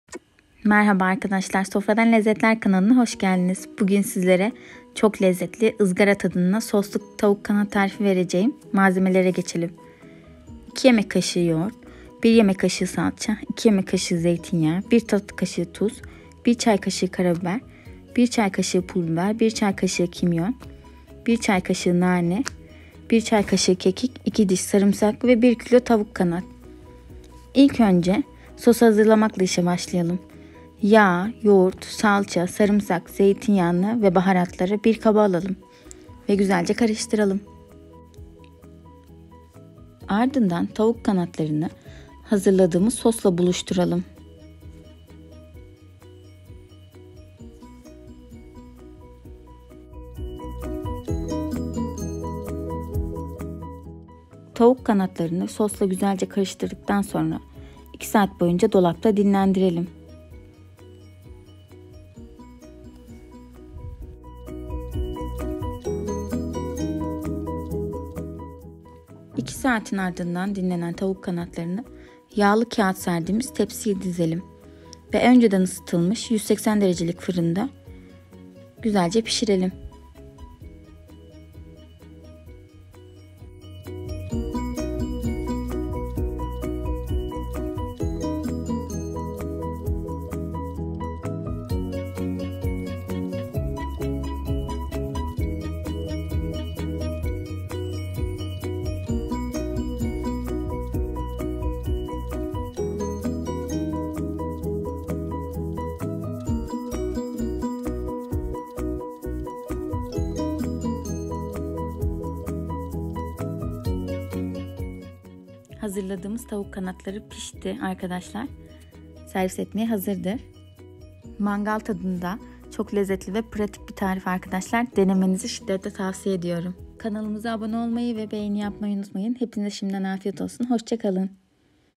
IZGARA TADINDA SOSLU TAVUK KANAT 🐔 | FAVORİNİZ OLACAK ÇOK LEZZETLİ BİR TARİF 😍😍
🌸Merhaba arkadaşlar tavuk kanat  sevenler
bu tarifte buluşuyor bir deneyin  favoriniz olacak yorumlarınızı bekliyoruz

MALZEMELER

2 yemek kaşığı yoğurt
1 yemek kaşığı salça
2 yemek kaşığı zeytinyağı
1 tatlı kaşığı tuz
1 çay kaşığı karabiber
1 çay kaşığı pul biber
1 çay kaşığı Kimyon
1 çay kaşığı nane
1 çay kaşığı kekik
2 diş sarımsak
1 kilo tavuk kanat